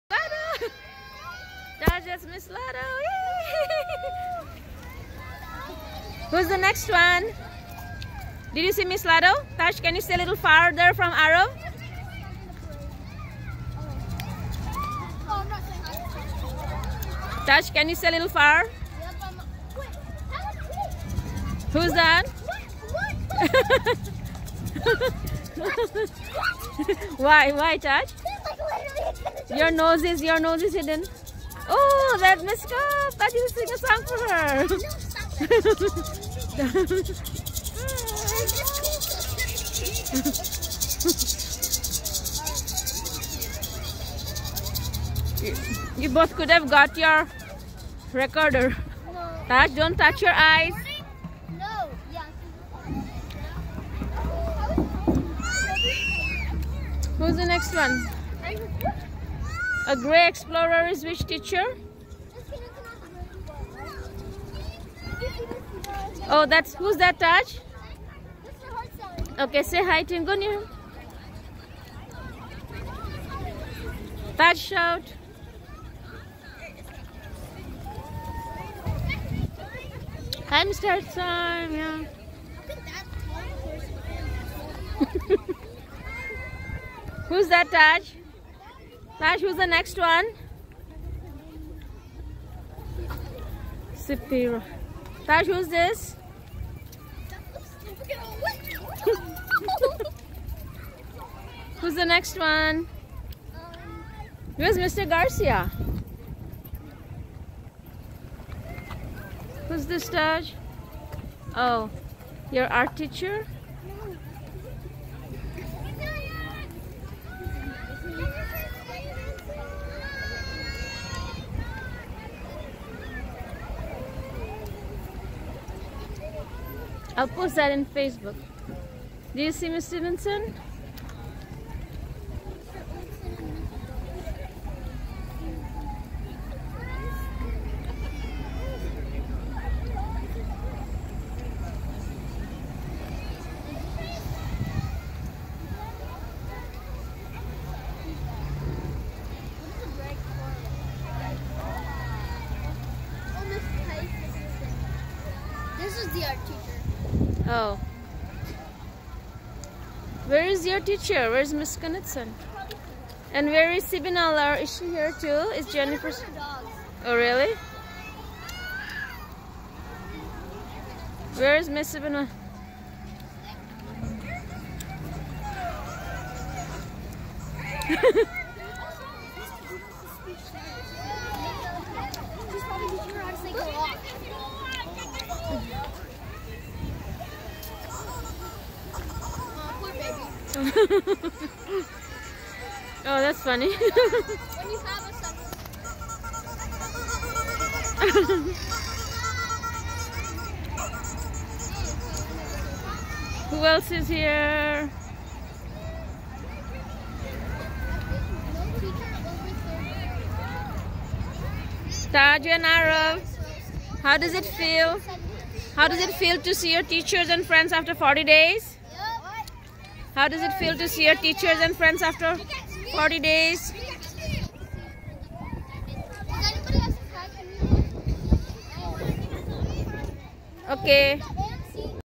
Cedar Park Staff Parade 2020 during COVID-19 Break
Cedar Park Staff did a fun car parade and met students and families at the AMC Parking area to show them how much they love and miss their students.